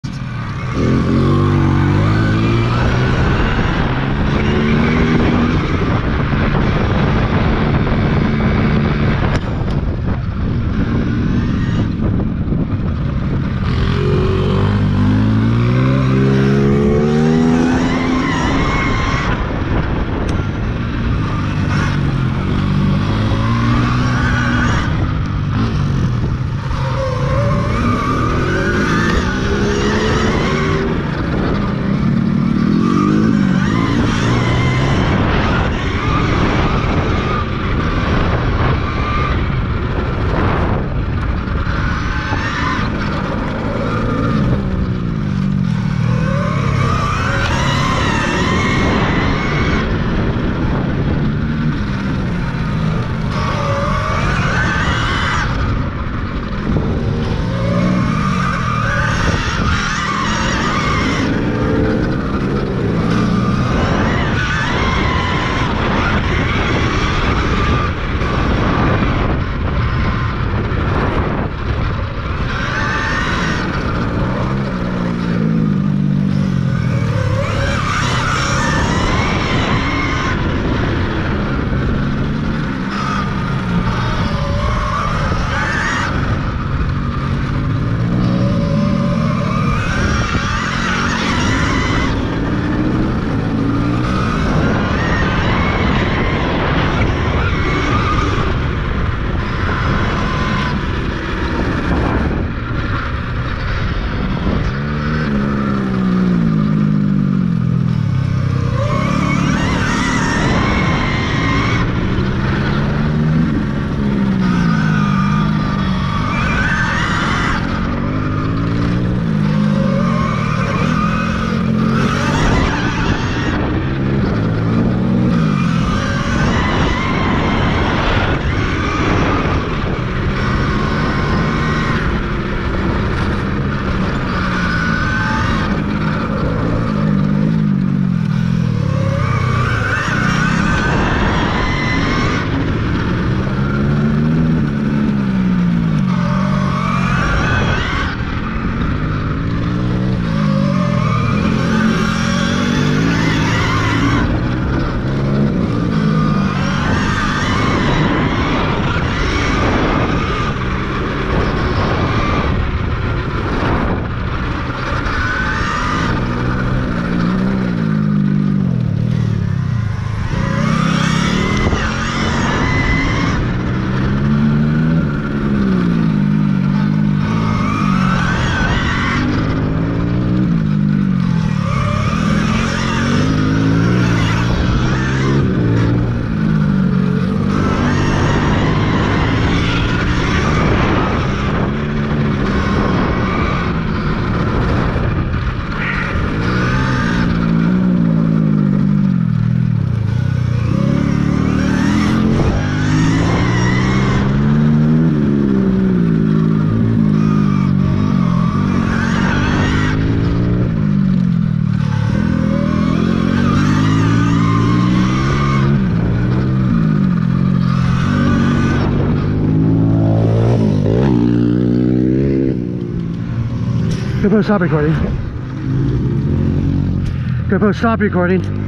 Dirt tracking at Brok McAllister's place, * The Big Track * 06/05/2023
This is me chasing around and filming Dave Bostrom. He lets me by and I do my best to hold him off,
I decided I would try to use the berm to finish-off the last turn, haven't quite perfected it yet. Good Times !!

Brock's place In Lincoln Ca.
With A.J., Dave Bostrom, A.J. Jr. Duke, Blair, Kevin, Brok, etc.